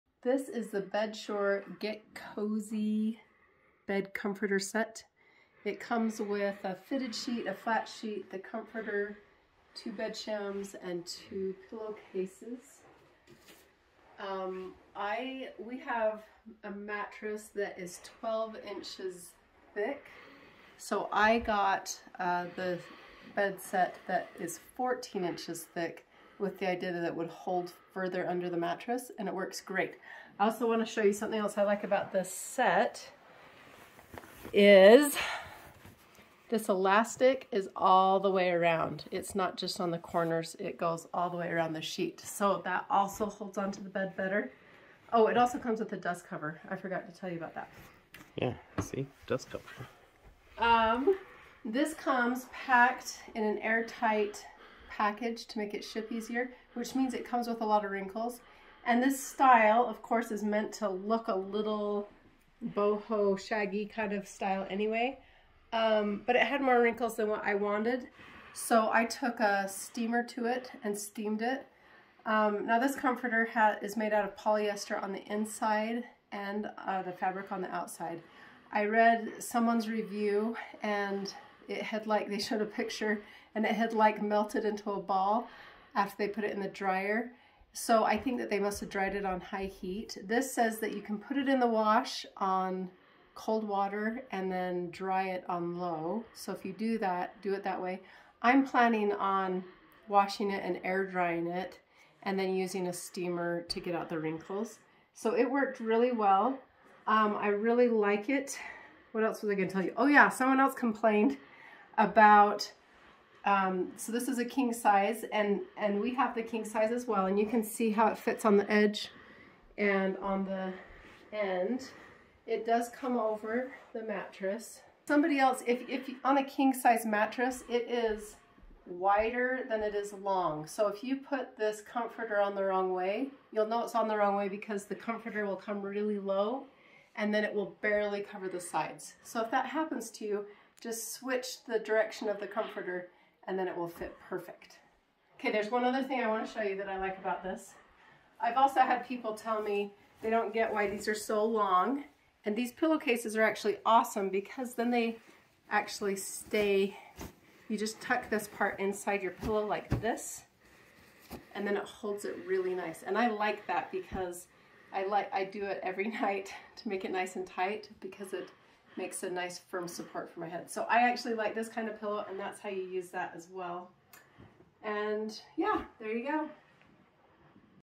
the Bedsure comforter and sheets set
We needed a new comforter set for our king size bed and decided to try out the Bedsure comforter and sheets set.

Overall, we're really happy with this set.  It came with wrinkles and needed to be steamed but after we did that we loved how it looked.  The comforter isn't quite as heavy as other comforters but it does keep us warm in the wintertime.  The fitted sheet has an elastic all the way around and fits our mattress really well.  Cool design, great set.